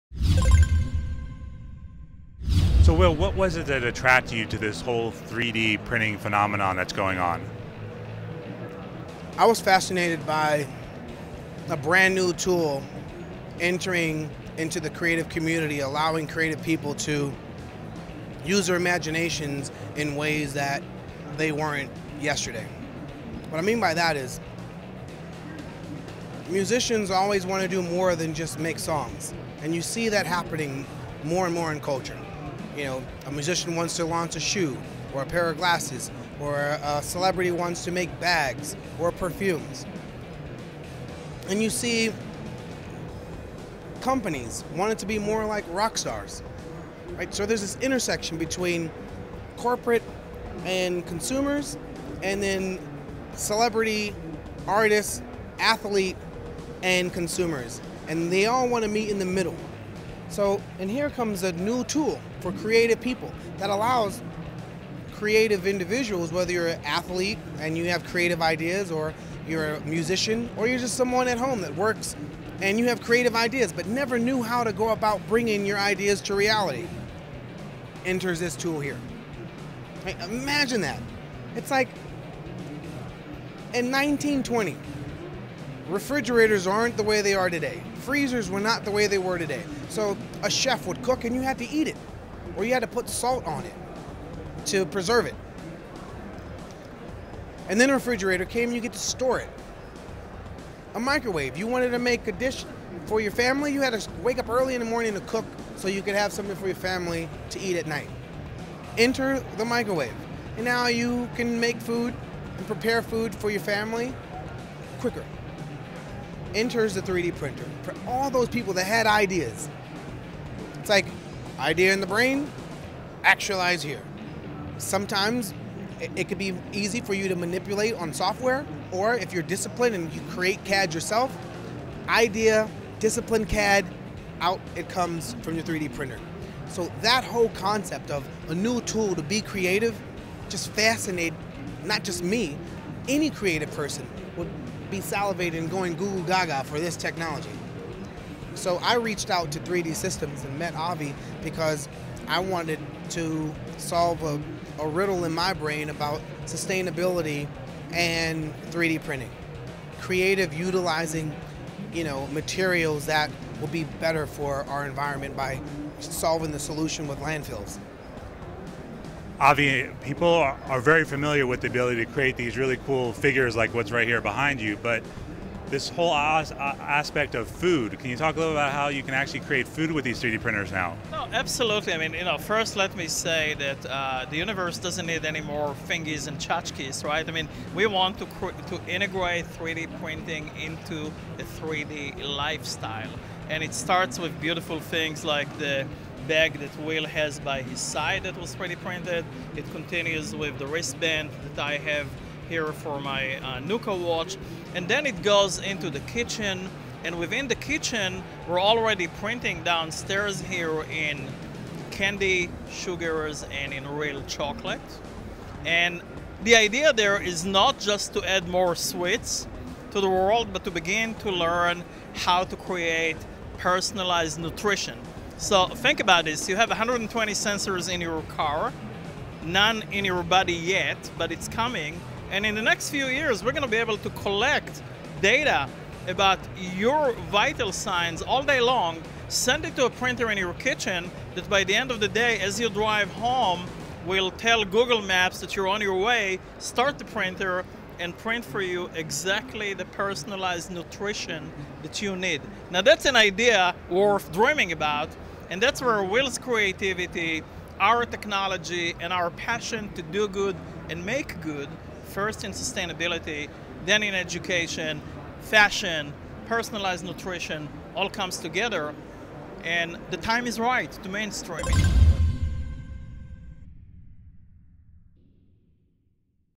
Will.i.am Talks 3D Printing
The Black Eyed Peas frontman, will.i.am, explains why 3D printing is the future in this exclusive interview from the 3D Systems booth at CES 2014.